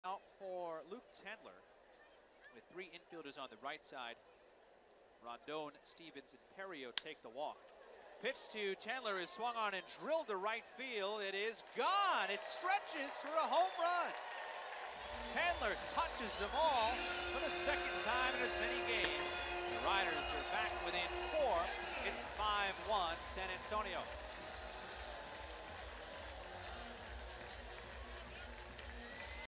Frisco's Tendler hits solo homer
6/14/17: Frisco's Luke Tendler connects on a solo home run in the fourth inning of a 6-3 loss to San Antonio